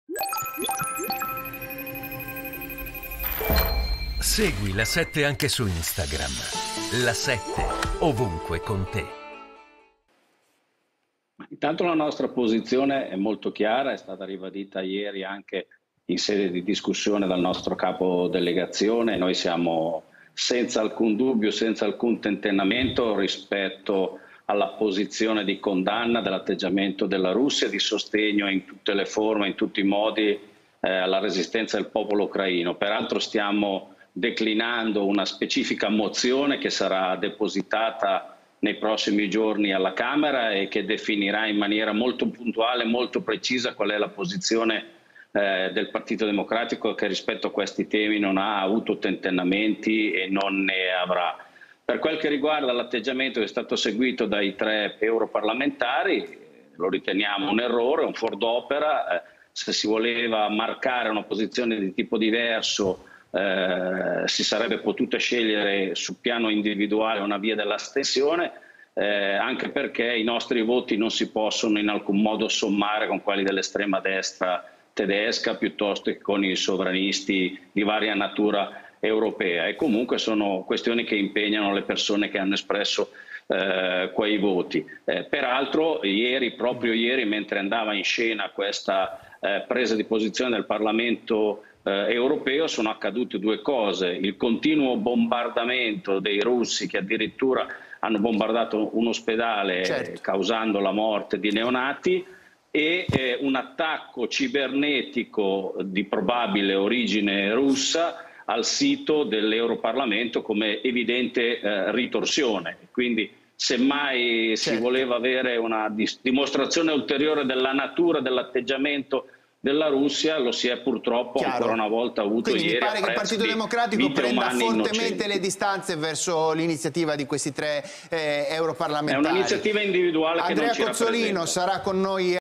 Enrico Borghi: "Condanniamo l'atteggiamento della Russia e sosteniamo in tutte le forme la ...
Enrico Borghi (PD): "Stiamo declinando una specifica mozione, che sarà depositata nei prossimi giorni alla Camera e che definirà in maniera molto puntuale la posizione del Partito Democratico".